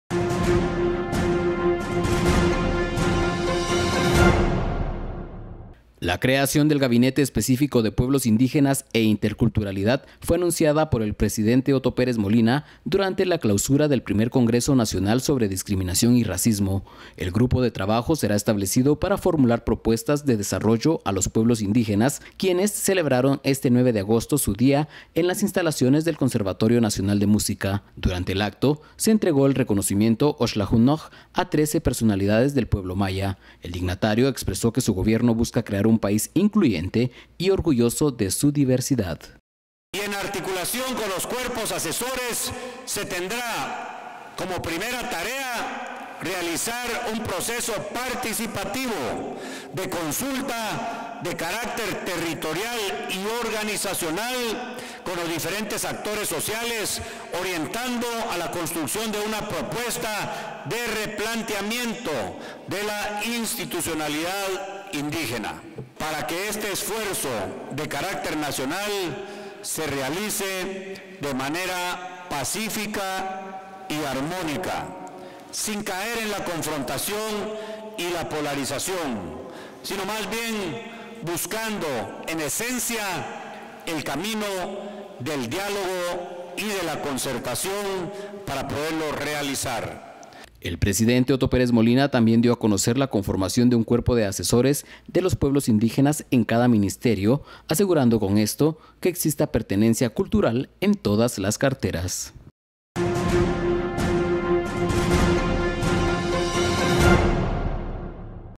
Crearán Gabinete de Pueblos Indígenas
Por Jarbin Yelmo/DCA. El presidente Otto Pérez Molina anunció durante la celebración del Día de los Pueblos Indígenas en el Conservatorio Nacional de Música, la creación de un Gabinete de Pueblos Indígenas, el cual estará conformado por un grupo de asesores indígenas en los ministerios de Gobierno. El mandatario entregó además el reconocimiento "Oxlajuj Na'Oj" a 12 líderes indígenas.